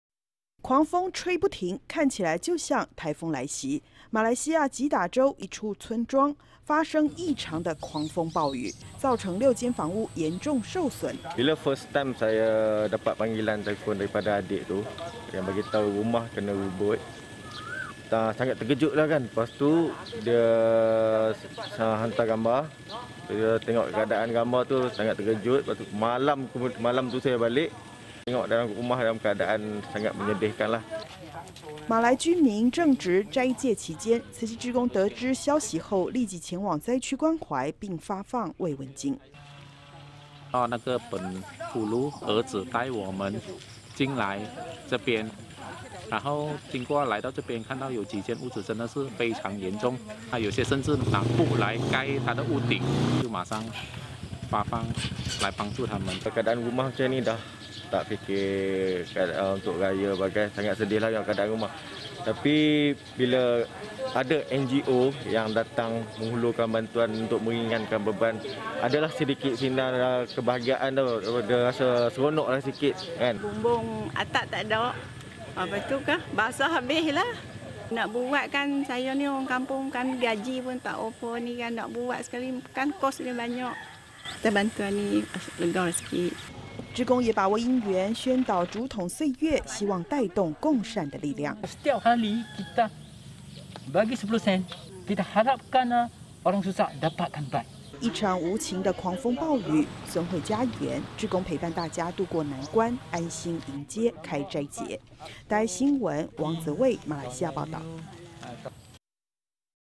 20220423【慈善】齋戒期暴雨毀家園 人間菩薩陪伴重建
受氣候變遷影響，馬來西亞最近發生瞬間大規模降雨的機率，越來越頻繁。位於吉打州一處村莊，最近(4/18)就發生狂風暴雨，共有6間房屋遭受嚴重破壞，屋頂被吹走，災民正值齋戒期間，志工得知消息後，前往現場關懷並送上慰問金。

狂風吹不停，看起來就像颱風來襲，馬來西亞吉打州一處村莊，發生異常的狂風暴雨，造成六間房屋嚴重受損。

受災戶 莫哈末穆斯林：「收到弟妹的電話，得知家裡被吹得非常嚴重，之後弟妹發了照片給我看，看到照片的情況，嚇到我了，趕緊從外地趕回來，看到家裡的情況，感到很傷心。」

馬來居民正值齋戒期間，慈濟志工得知消息後，立即前往災區關懷，並發送慰問金。

慈濟志工 陳可文：「那個村長的兒子帶我們進來這邊，經過來到這邊看到幾間屋子，真的是(受損)非常嚴重，有些甚至是拿布來蓋他的屋頂，就馬上發放來幫助他們。」

受災戶 莫哈末穆斯林：「家裡變成這樣，已經無心過節了，但是慈濟前來伸出援手，幫助我們減輕負擔，度過眼前難關， 讓我們的壓力沒那麼大了，放鬆了一些，也覺得很幸福。」

受災戶 奴瑪瓦蒂：「我家的天花板飛了，家裡全泡水，如果要維修，我們的工資也不夠，如果要一次全部修好，費用很高，慈濟幫了我們，可以鬆一口氣。」

志工也把握因緣，宣導竹筒歲月，希望帶動共善的力量。

慈濟志工 陳如德：「每天我們放10分錢，我們希望可以幫助更多有需要被幫助的人。」

一場無情的狂風暴雨，損毀家園，志工陪伴大家度過難關，安心迎接開齋節。

王哲蔚 馬來西亞報導